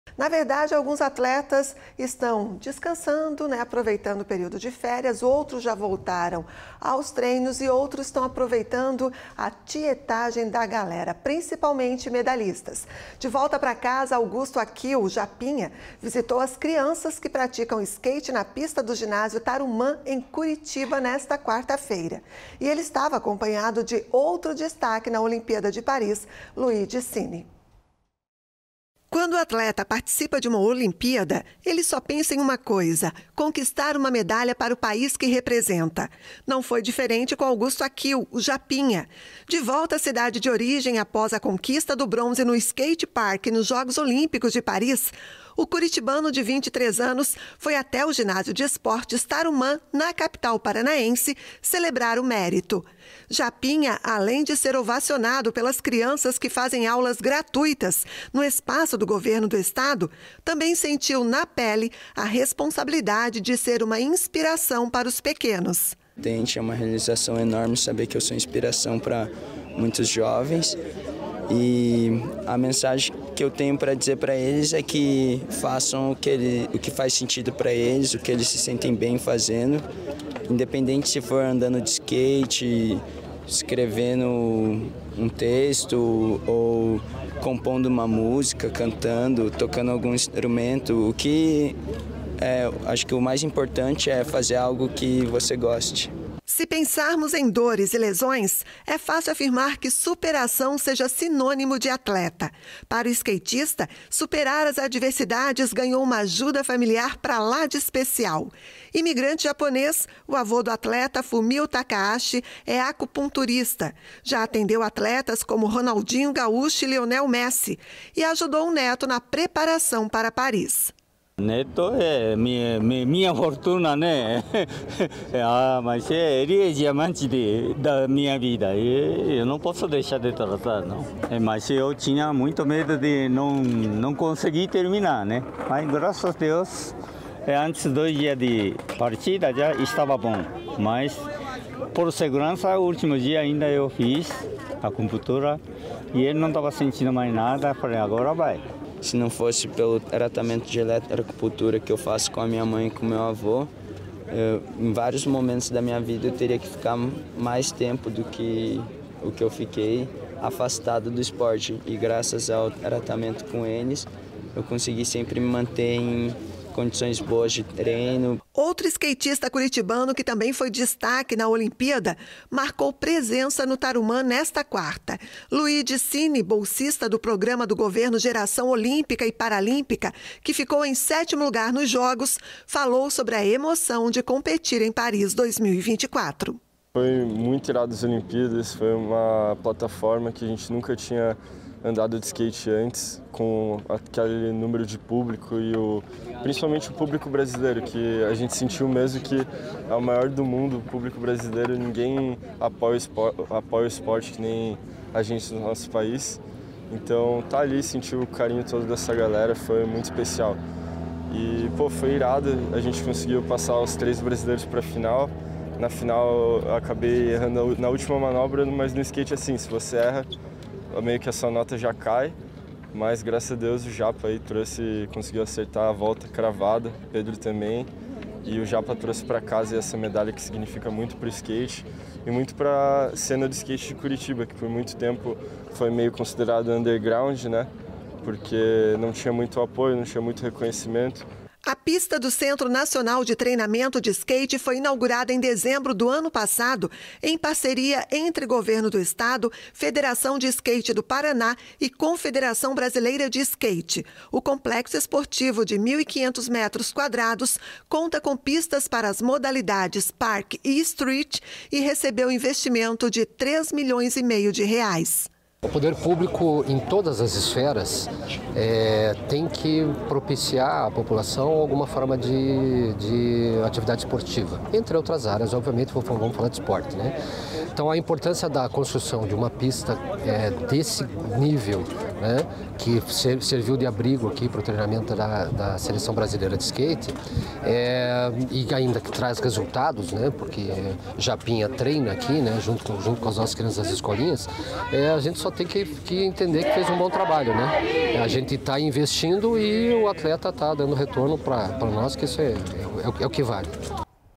Japinha: bronze no Skate Park visita ginásio em Curitiba
De volta pra casa, Augusto Akio, o Japinha, visitou as crianças que praticam skate na pista do ginásio Tarumã, em Curitiba, nesta quarta-feira (14). Ele estava acompanhado de outro destaque na olimpíada de Paris, Luigi Cini.

  / pixtvhd
  / pixtvhd
  / pixtvhd

🅿️ℹ️❎
PIX NEWS